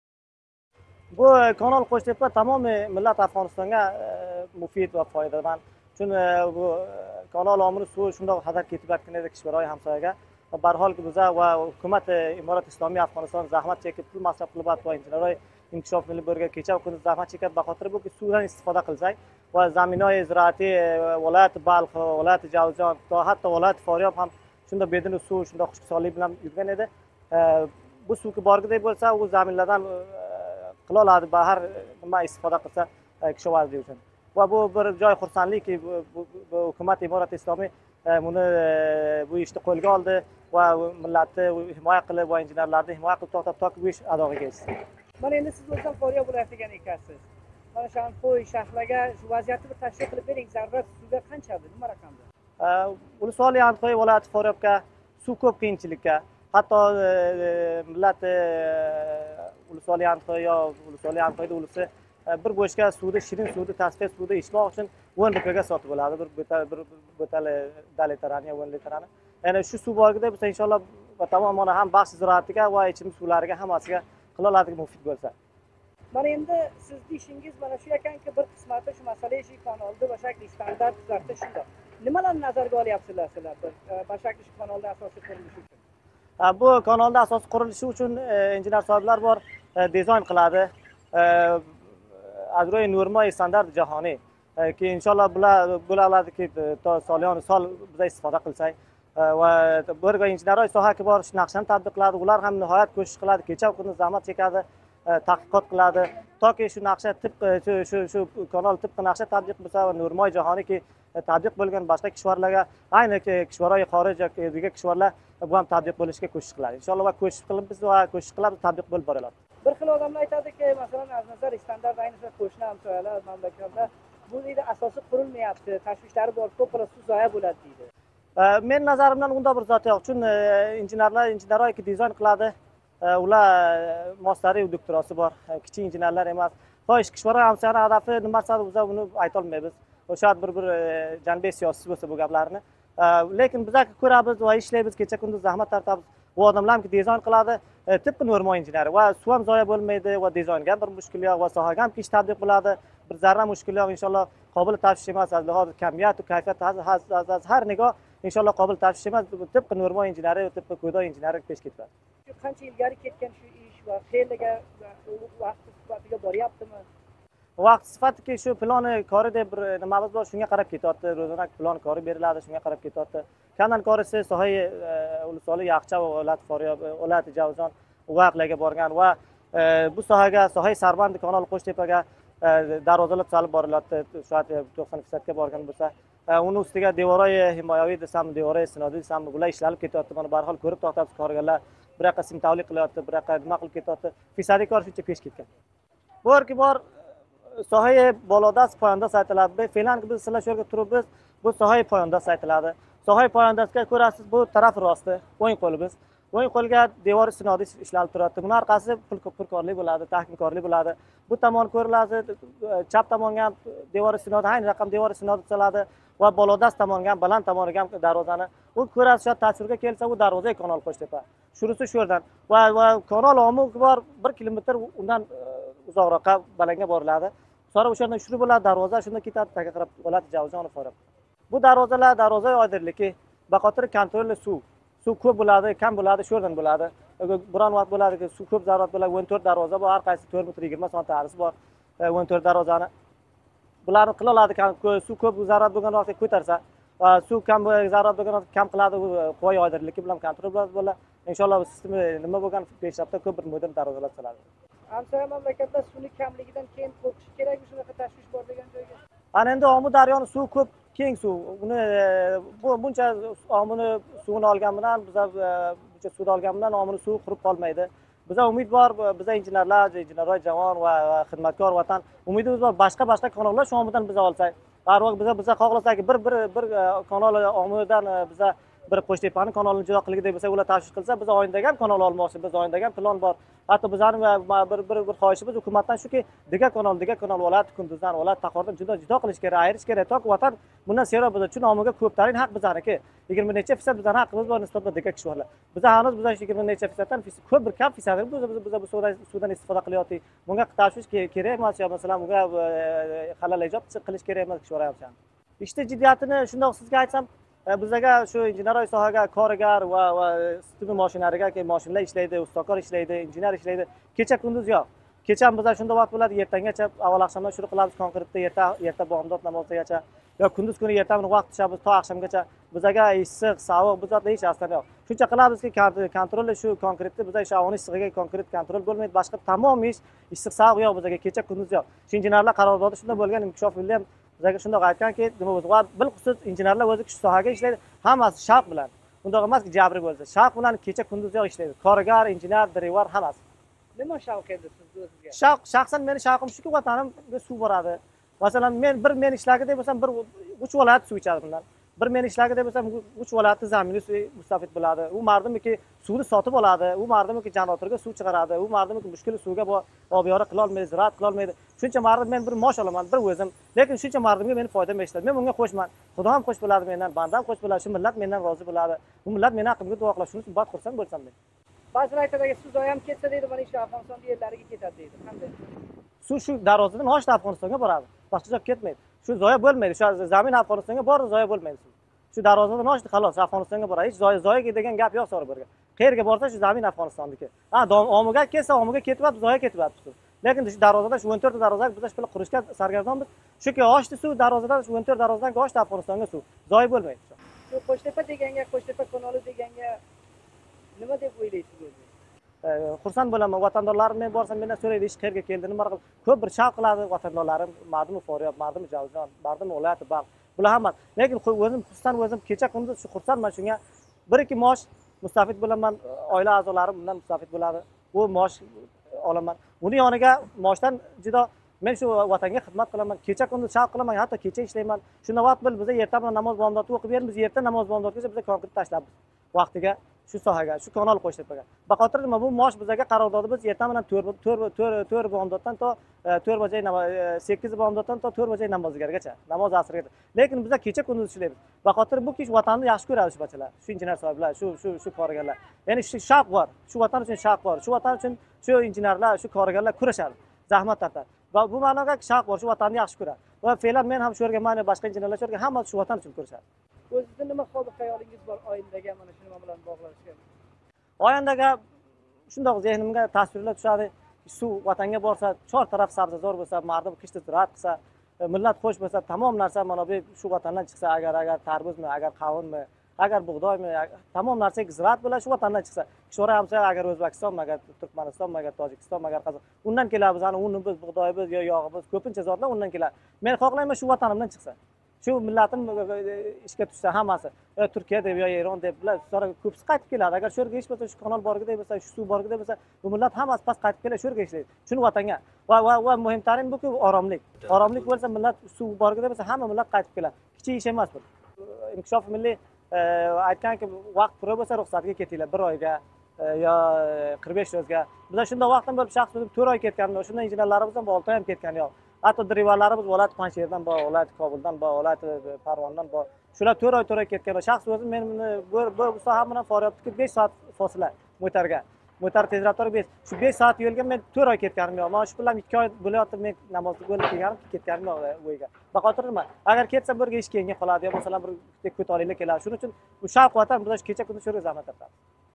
قوش تېپه‌ده ایشلر جدل بارماقده
مهندس نورالله بونی وطنگه خدمت دېیدی